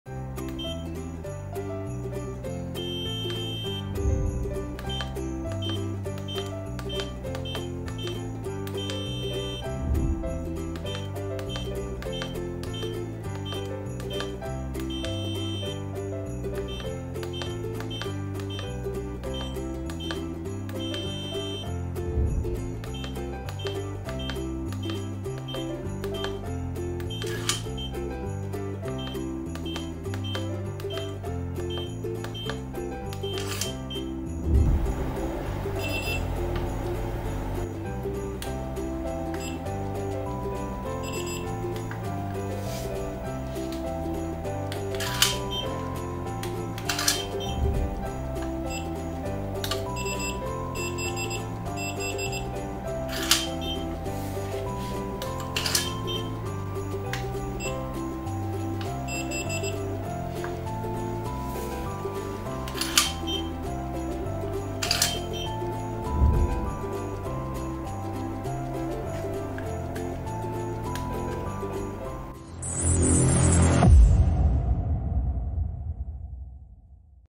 MICTUNING Keyless RV Entry Door Latch Digital Lock
10 Digits LED Keypad, Wireless Key Fob, Electronic NFC & Mechanical Key

Today's video will show you how to set new password?
 how to reset to factory setting and re-pair the KeyFob and NFC Key?